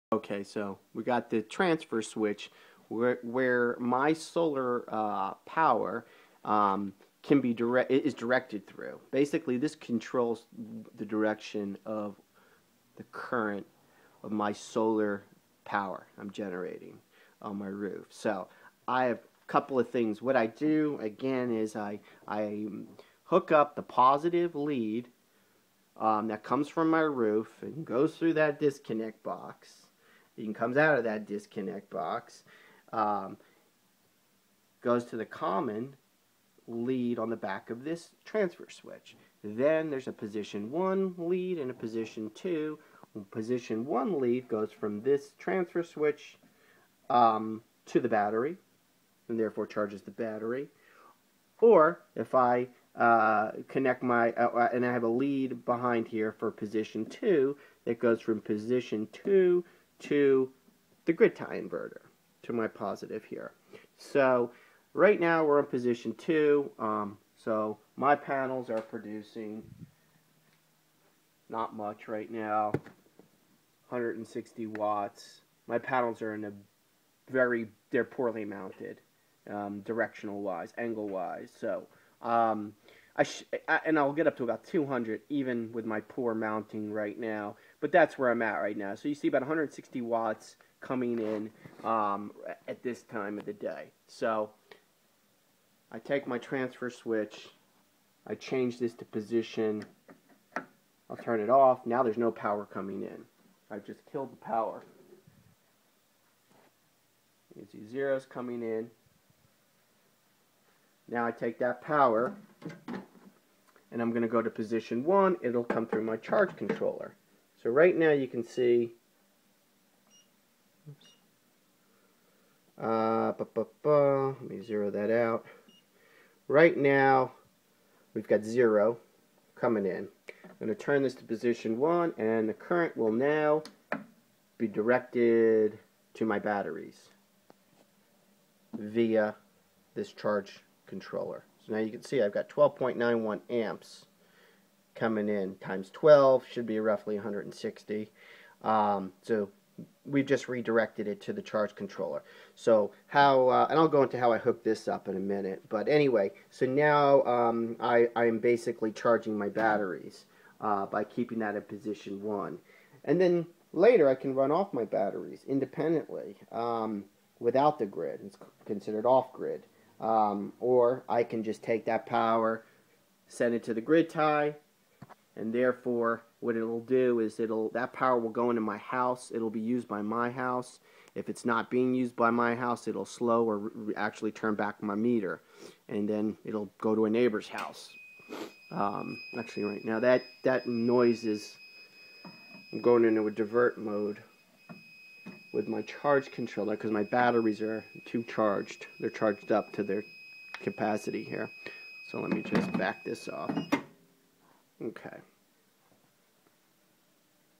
Solar system breaking it down #2
Discuss break down of solar components and transfer switch